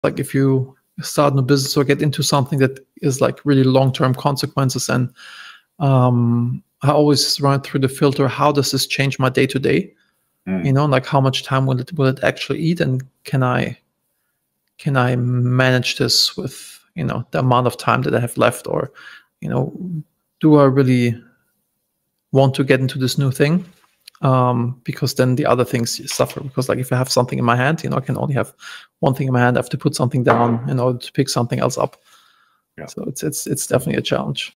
Navigating the Commitment Conundrum: Weighing Time Investment in Business
Success isn't just about the big picture - it's about managing the day-to-day too. Before diving into long-term commitments, I always ask: How will this impact my daily routine? It's about finding balance and making informed choices for lasting success.